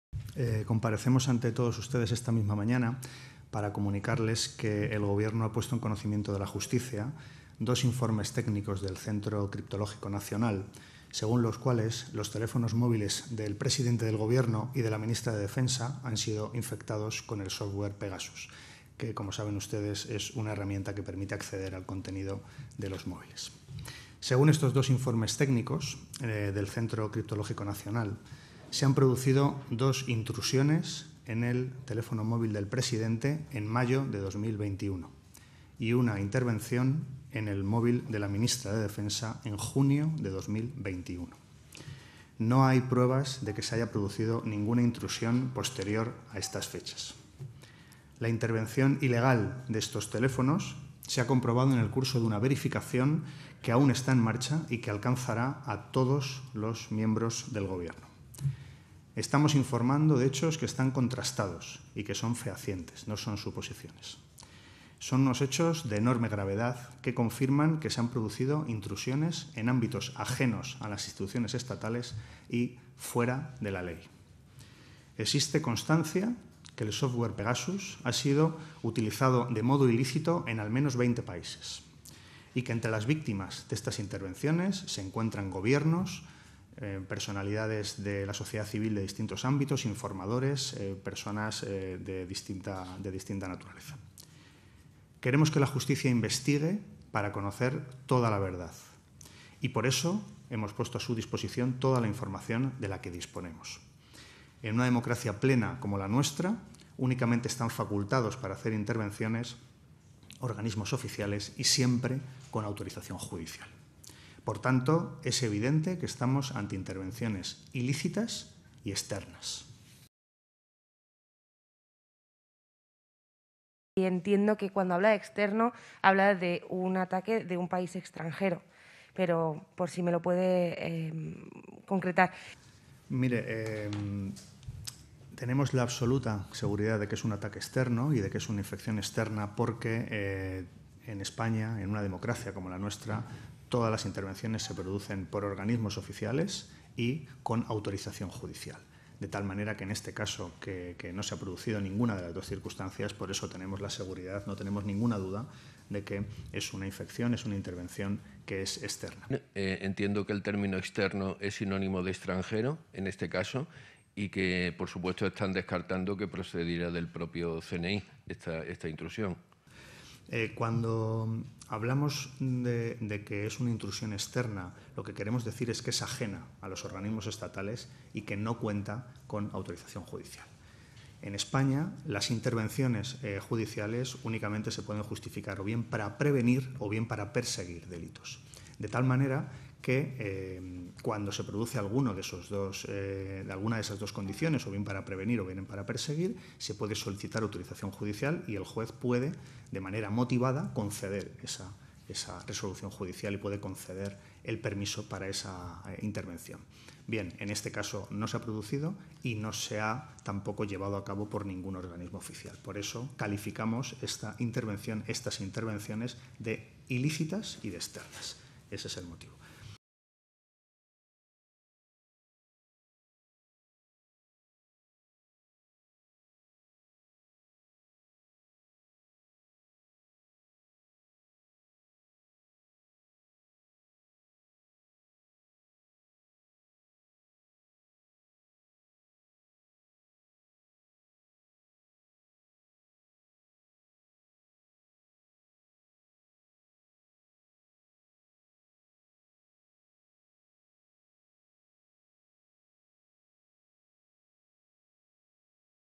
El móvil de Robles y Sánchez INFECTADO CON PEGASUS y fue un ataque externo PORQUE LO DICE BOLAÑOS
No lo sé Rick, van a tener que sacar más pruebas que "en España no se espía" para convencerme de que no fueron o las cloacas o el propio PsoE